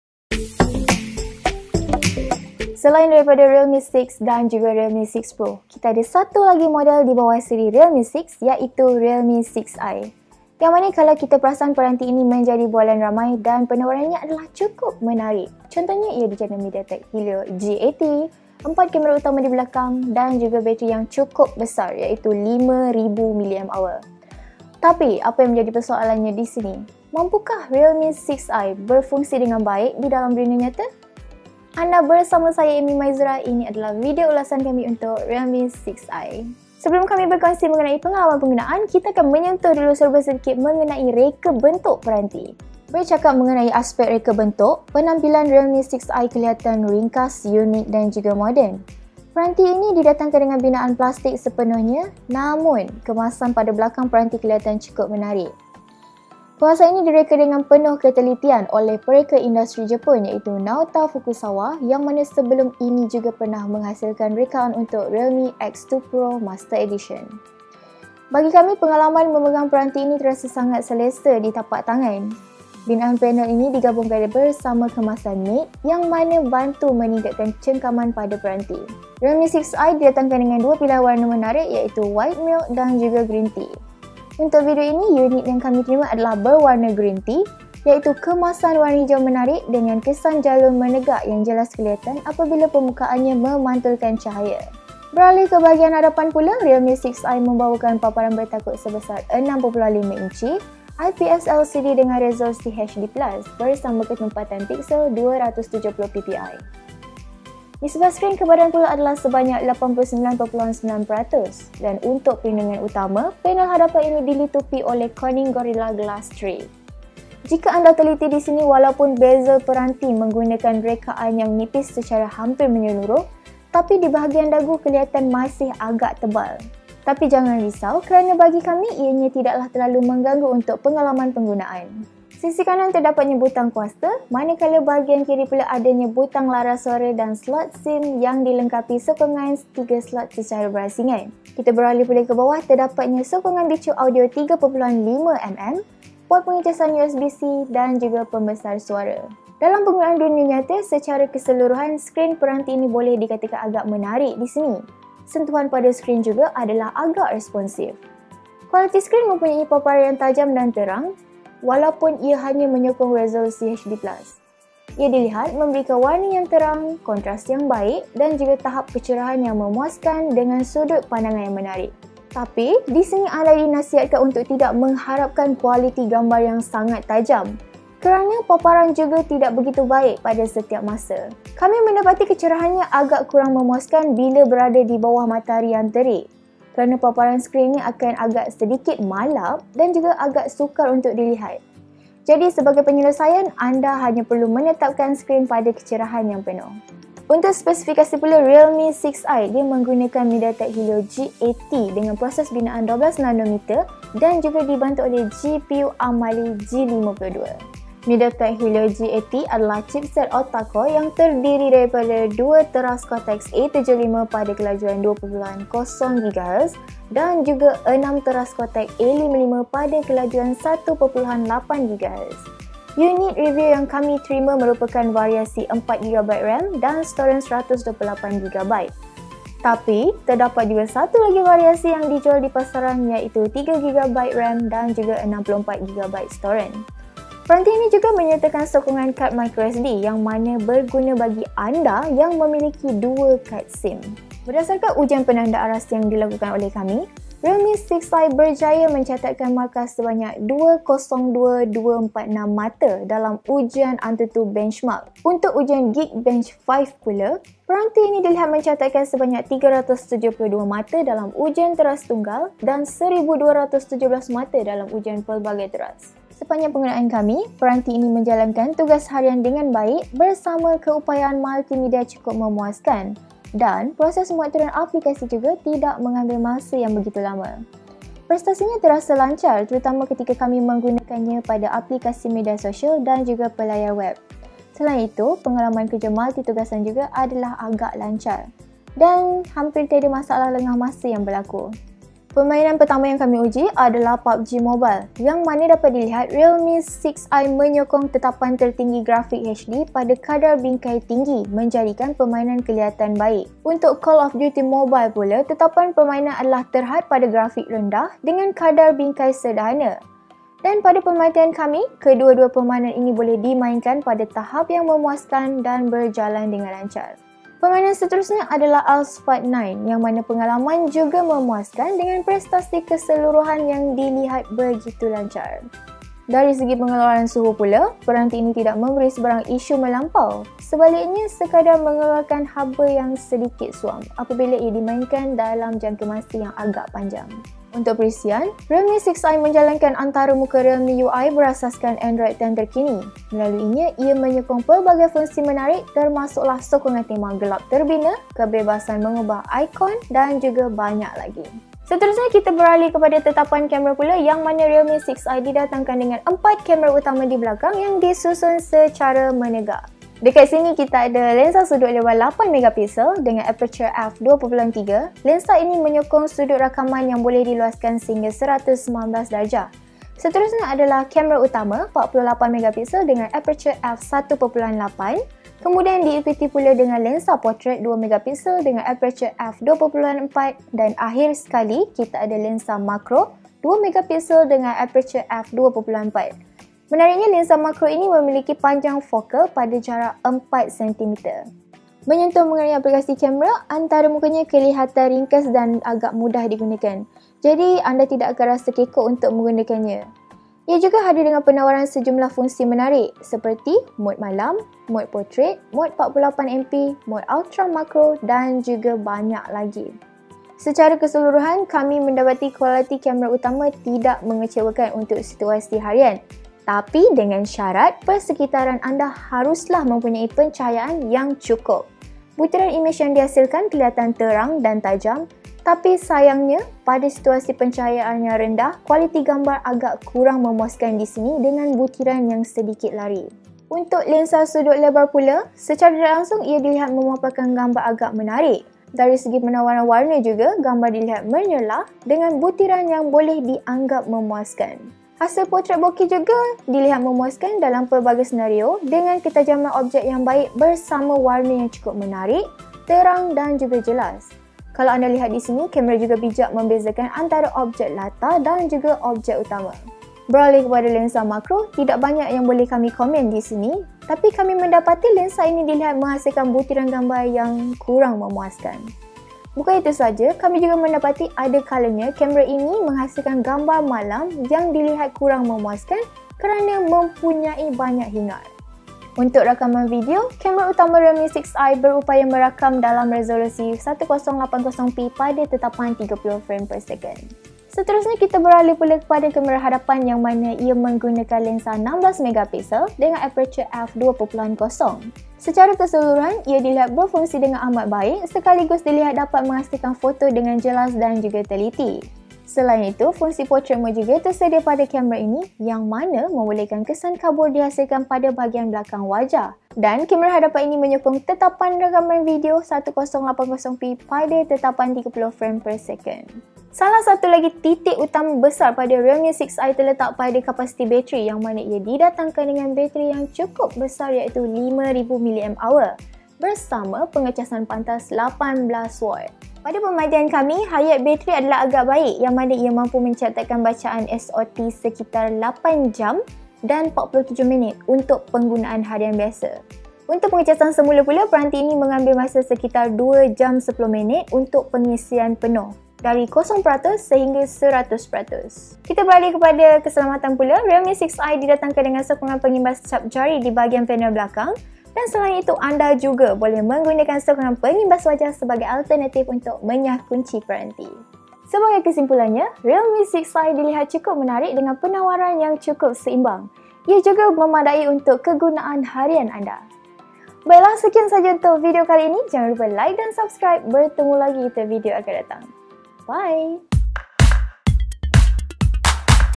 Ulasan : Realme 6i
Realme 6i merupakan antara peranti yang terbaru ditawarkan dalam siri realme 6. Apa yang menarik mengenai peranti ini adalah ia mempunyai penawaran quad kamera belakang antaranya adalah 64 Megapixel. Cip pemproses Helio G80, memori 3 hingga 4GB RAM dan storan dalaman 64 dan 128GB bermula pada harga RM699.